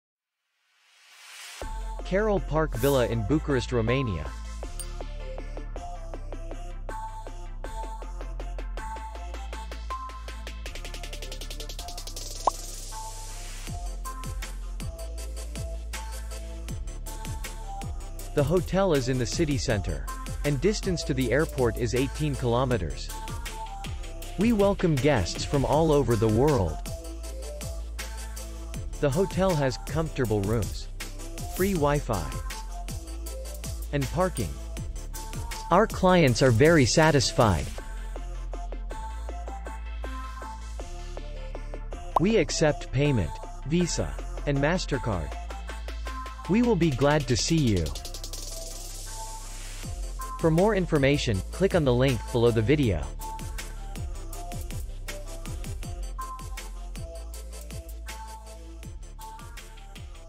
Carol Park Villa hotel review | Hotels in Bucharest | Romanian Hotels
Carol Park Villa hotel video review. Hotels in Bucharest, european 3 stars hotels.